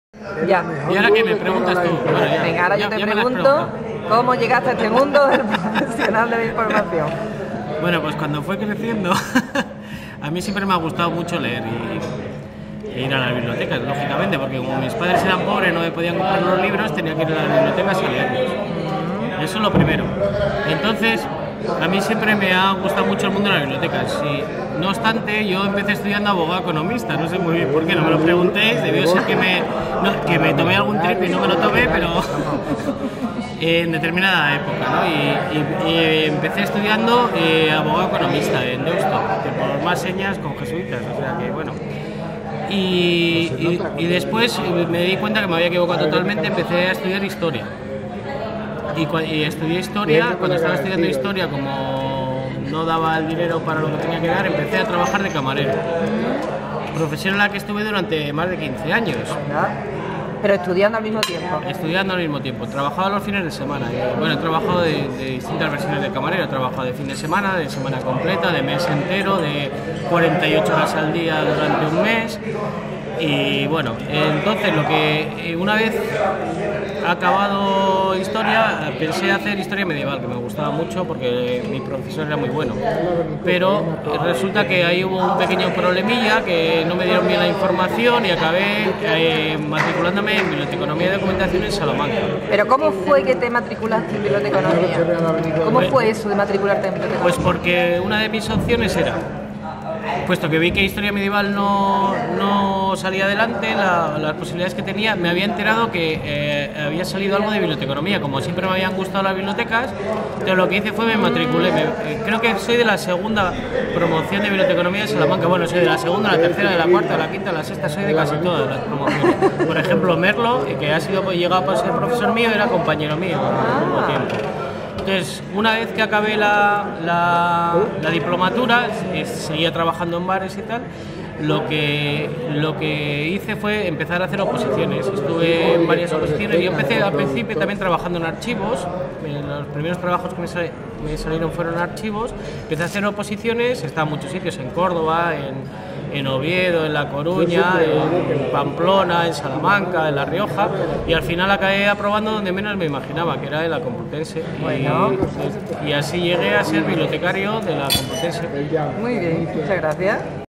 ¿Por qué soy bibliotecario? Andoni Calderón
Bibliotecario de la Universidad Complutense de Madrid